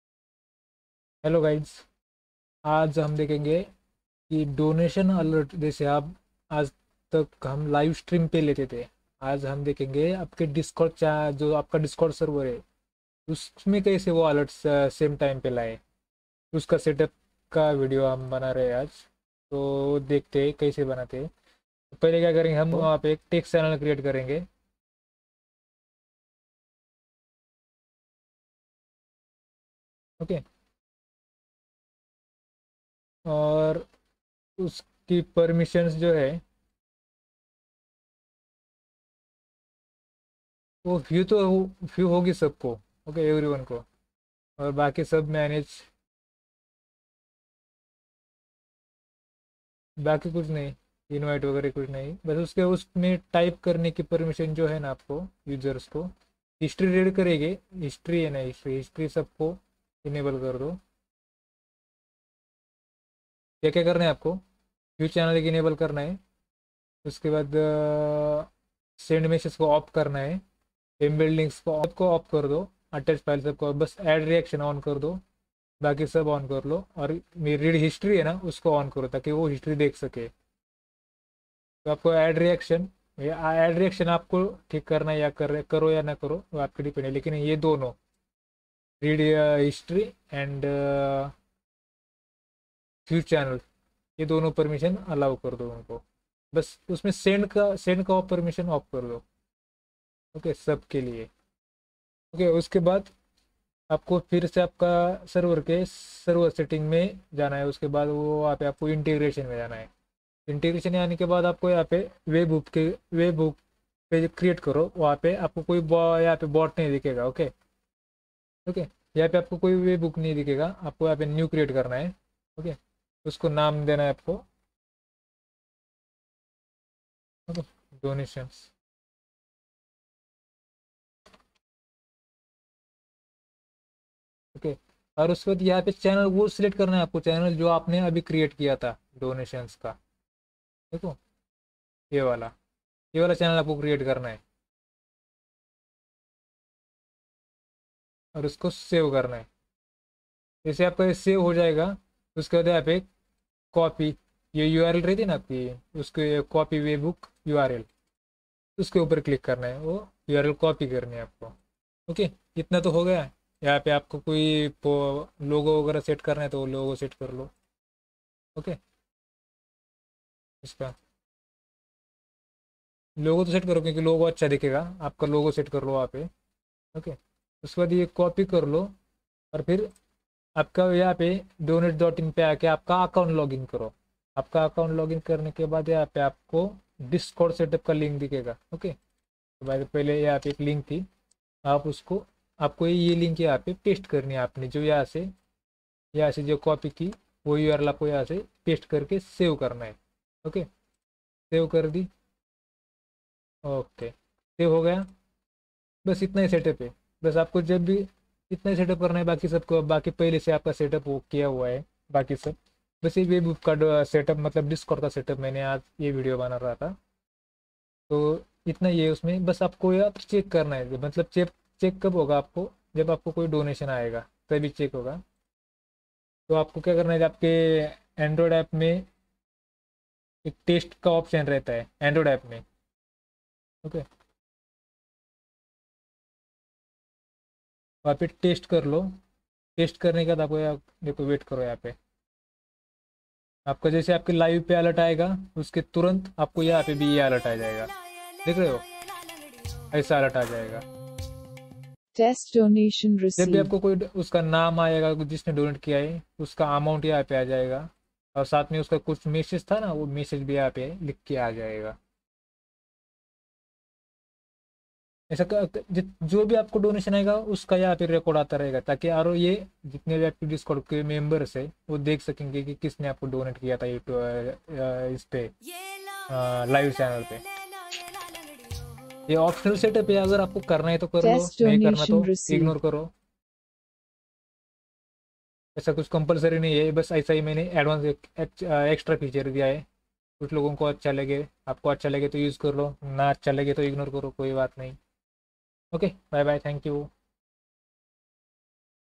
Get Donation alert on your discord | PayTM GooglePay Phonepe Alert on Discord and Live stream
Show PayTM GPay PhonePe BHIM donation on live stream free setup video